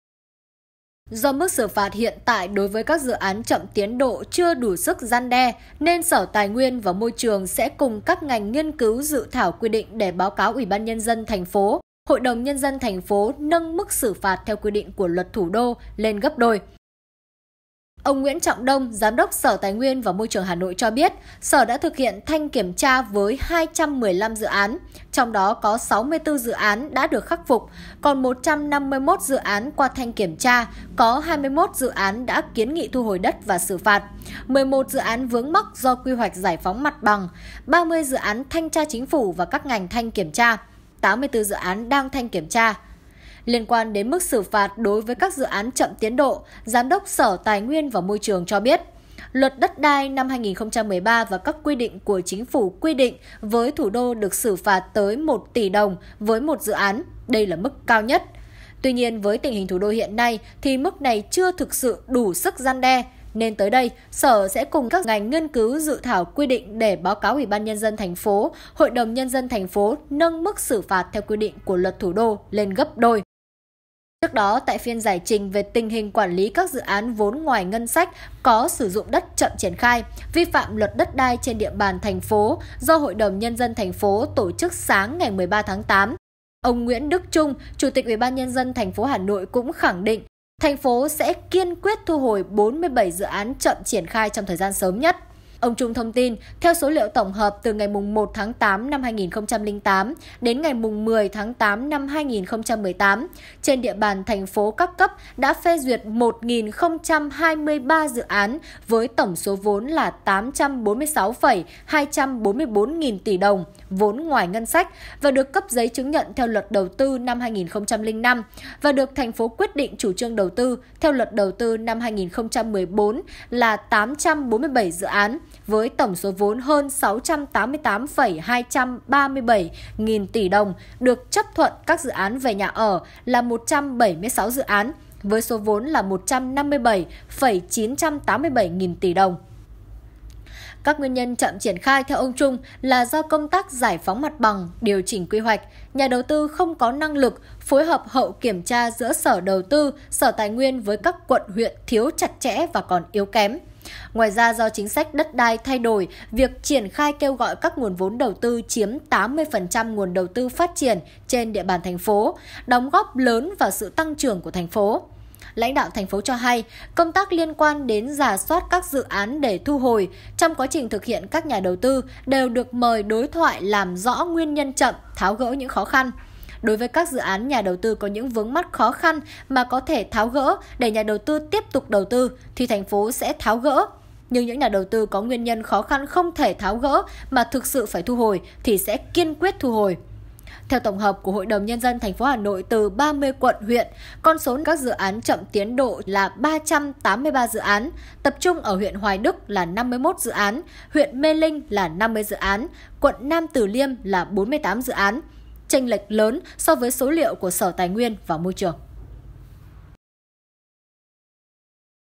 Hà Nội sẽ tăng gấp đôi mức xử phạt dự án chậm tiến độ?
Do mức xử phạt hiện tại đối với các dự án chậm tiến độ chưa đủ sức răn đe nên Sở Tài nguyên và Môi trường sẽ cùng các ngành nghiên cứu dự thảo quy định để báo cáo UBND TP, HĐND TP nâng mức xử phạt theo quy định của Luật Thủ đô lên gấp đôi, ông Nguyễn Trọng Đông - Giám đốc Sở Tài nguyên và Môi trường Hà Nội cho biết.